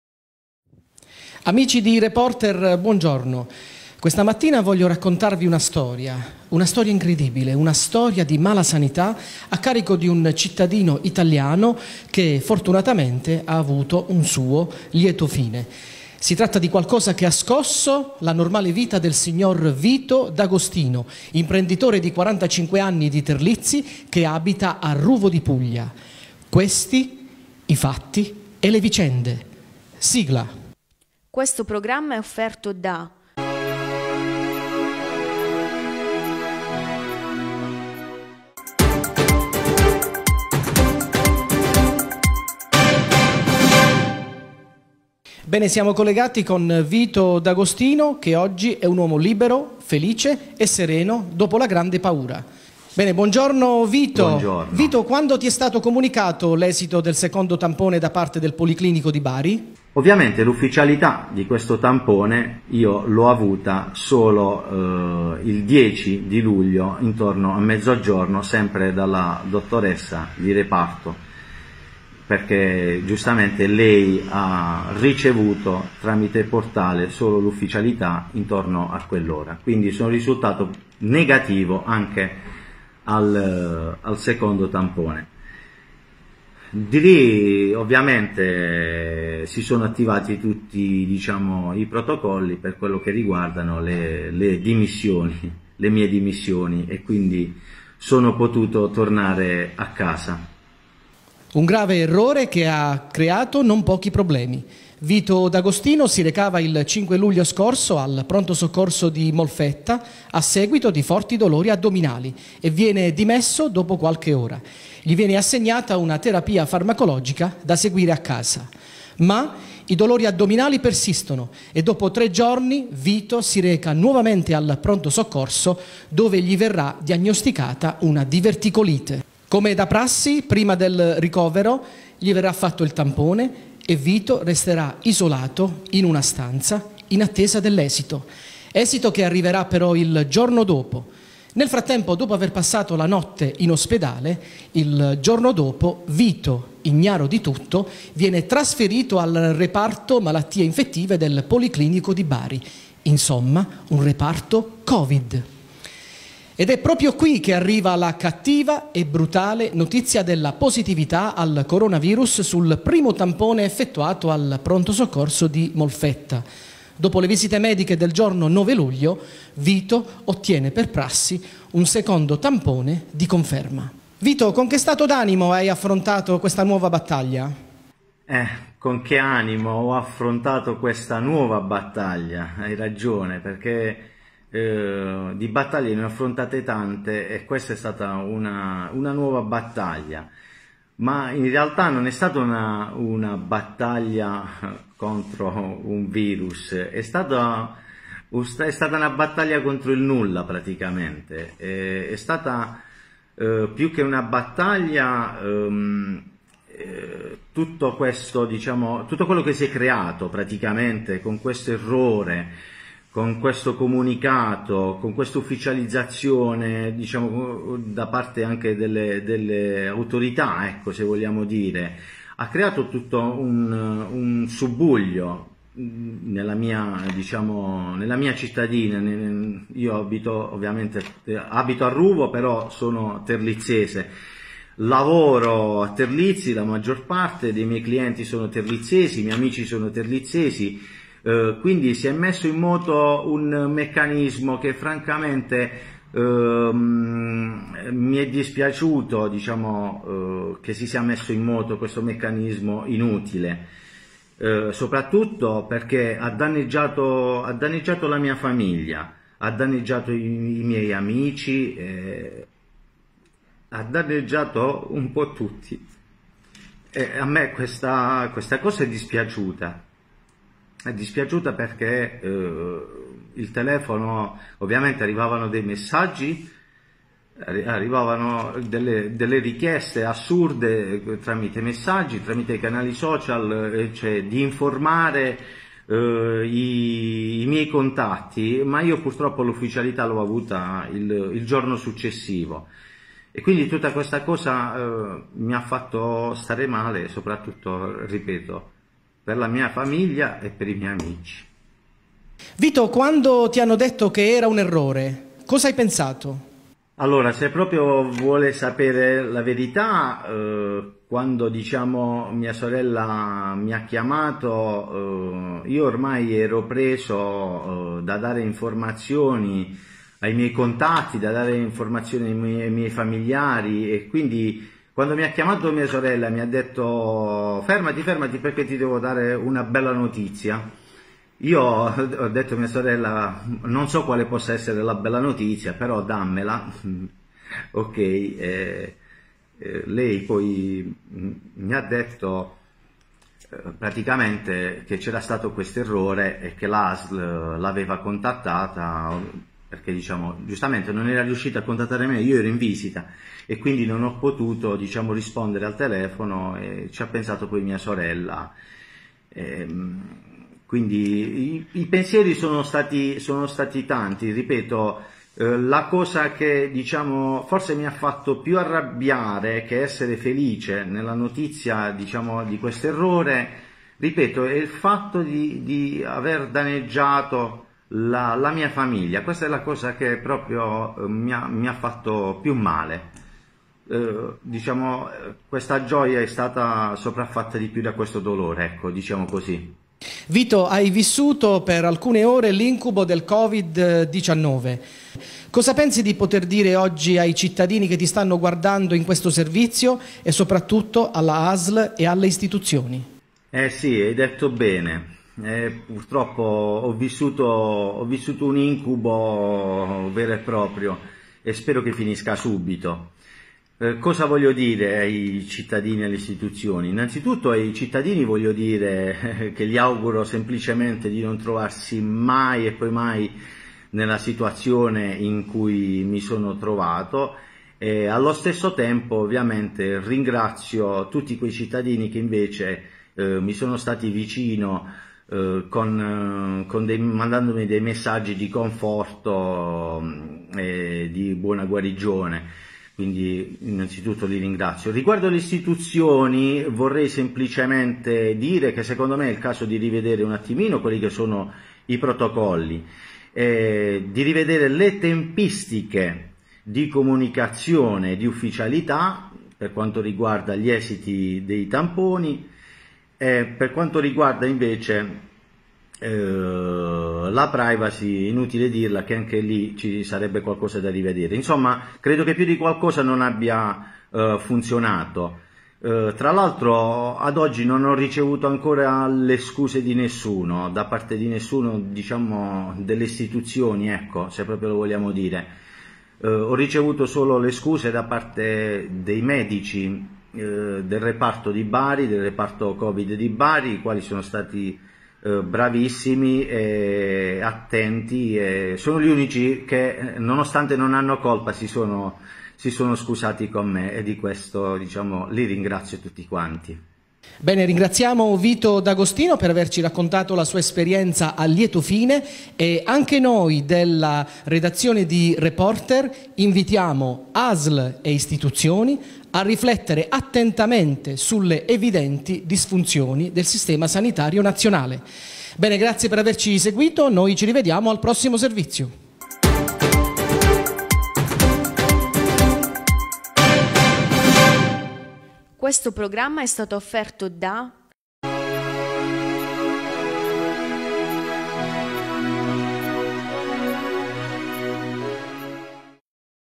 MALASANITA': errore di trascrizione e drammi umani. Servizio a cura di Domenico Lobascio
"CLAMOROSO ERRORE DELLA ASL": INFETTO AL COVID19 PER UN GIORNO E MESSO IN QUARANTENA FIDUCIARIA PER UN ERRORE DI TRASCRIZIONE. INCUBO FINITO? - Servizio a cura di Domenico Lobascio - intervista integrale all'imprenditore Vito D'Agostino